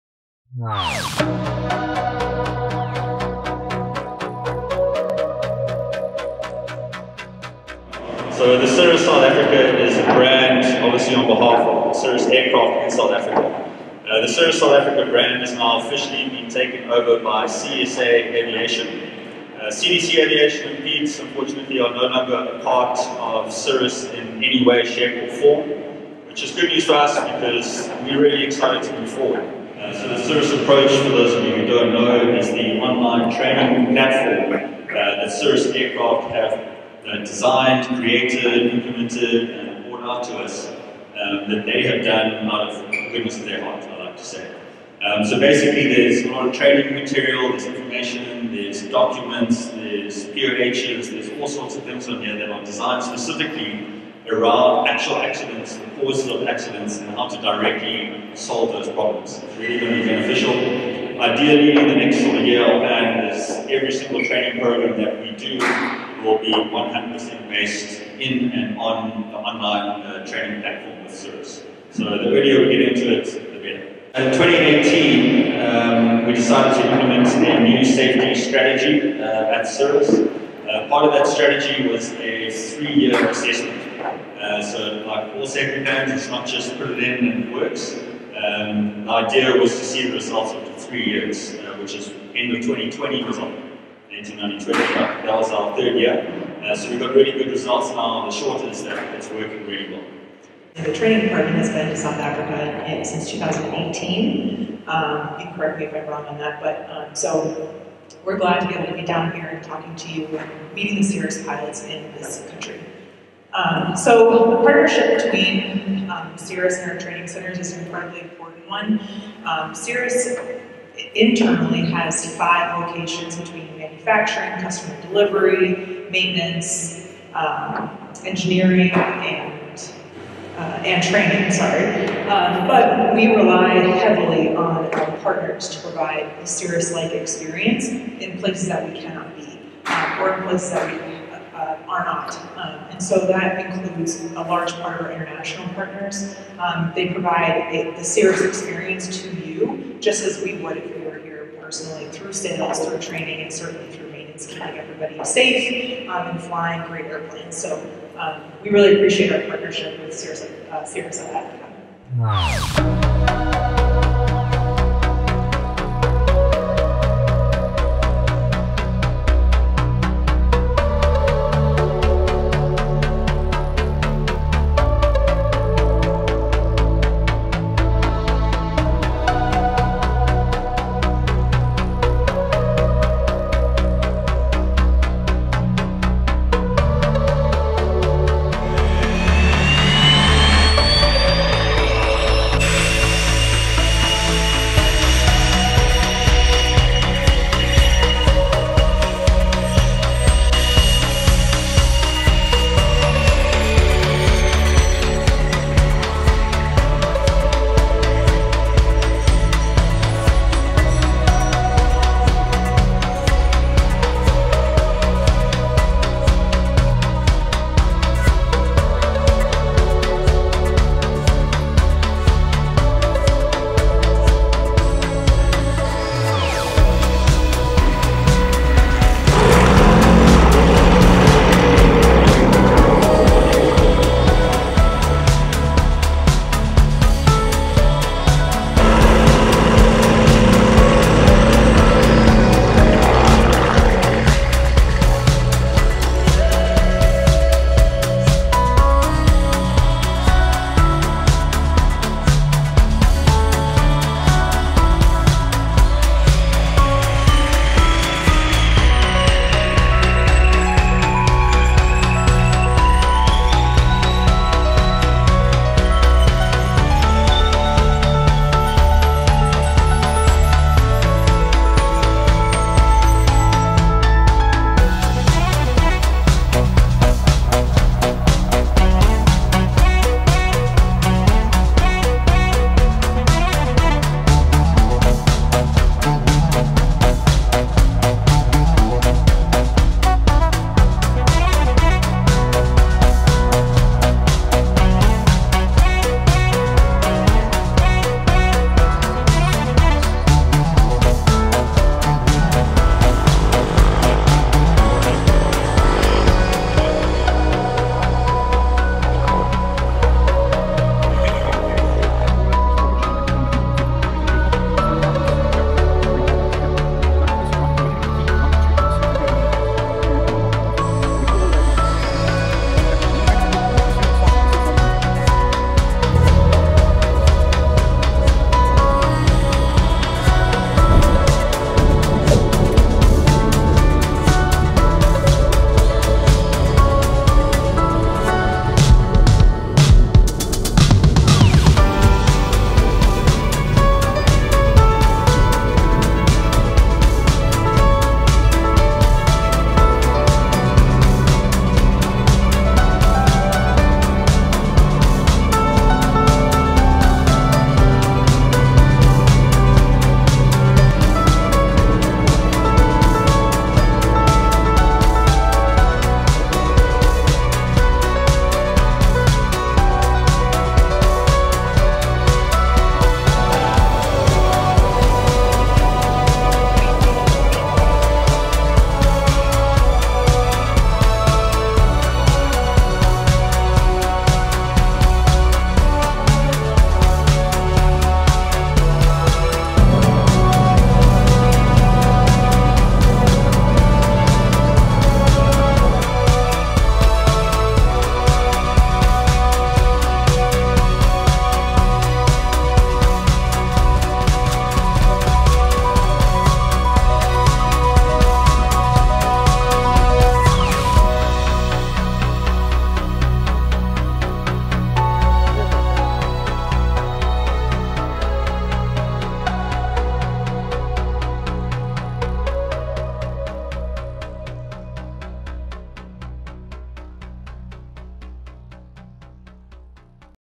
Cirrus Aircraft - South Africa
Over the past month, I worked closely with Cirrus Aircraft South Africa as part of the
launch of the new Cirrus aircraft representation in southern Africa. On Friday evening 13
August Cirrus held its traditional safety seminar which is dedicated towards the correct
training for ALL Cirrus pilots. The international Cirrus training representative Amy Voss
presented a brilliantly illustrated lecture on the benefits of the Cirrus system, especially
the CAPS parachute deployment when things go wrong.

AFRICAN PILOT MAGAZINE